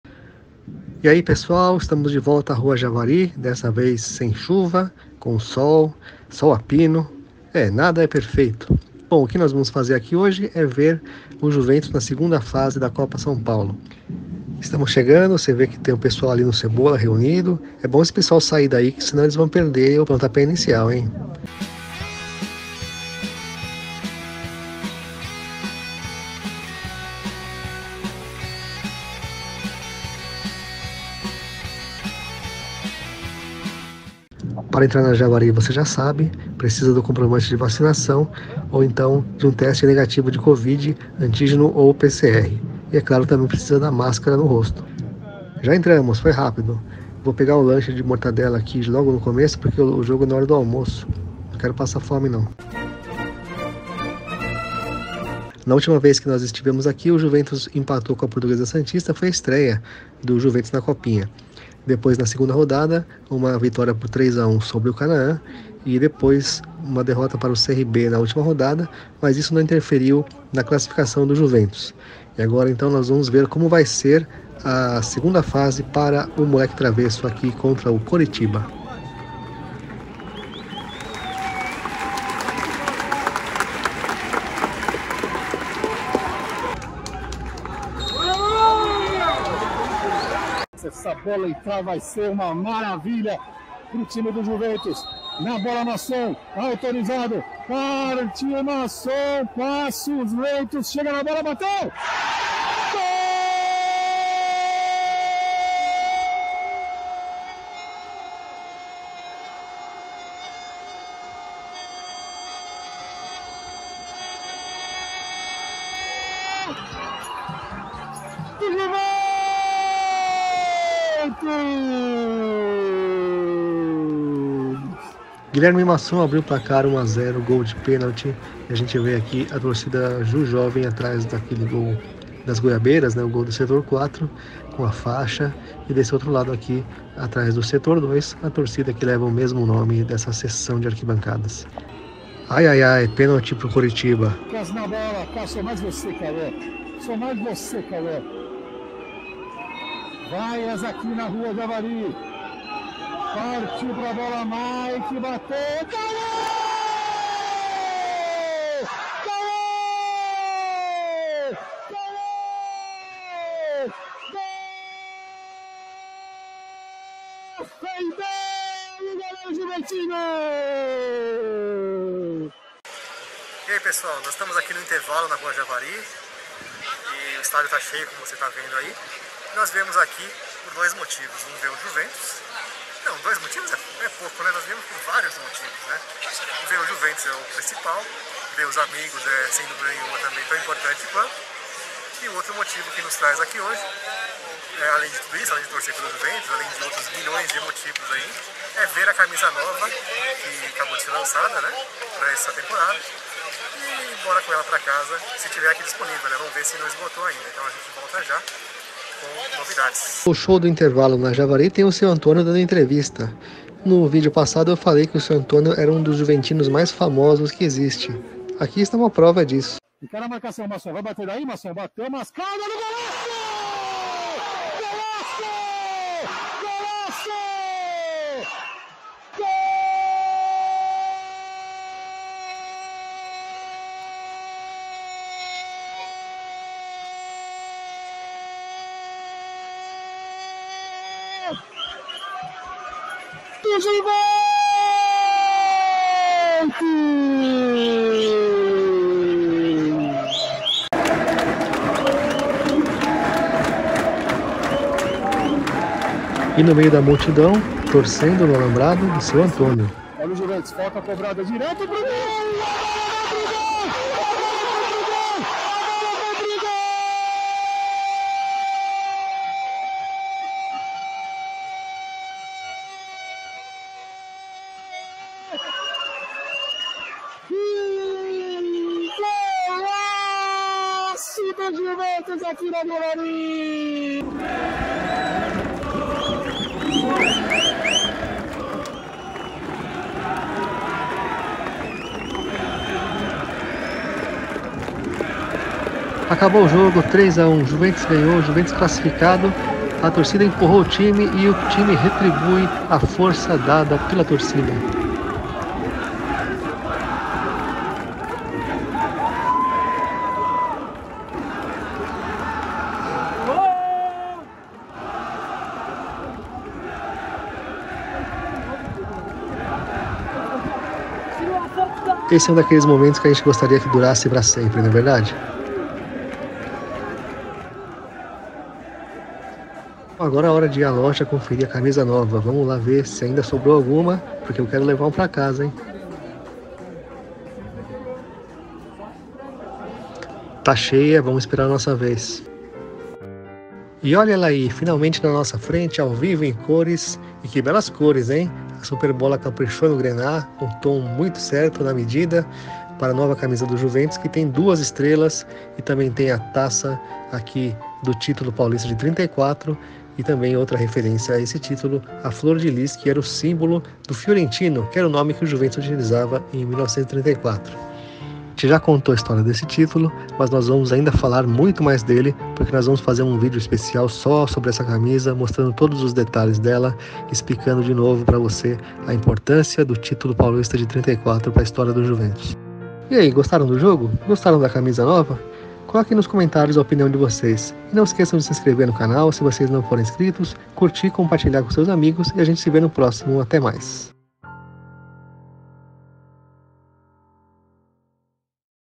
Juventus bate o Coritiba na Copinha | A nova camisa 2022 do Juventus | Manto Juventino
Pela segunda fase da Copa São Paulo, o Juventus venceu o Coritiba por 3 a 1. Aqui você confere as cenas que registramos e também a nova camisa para 2022 lançada e já à venda nas lojas do clube.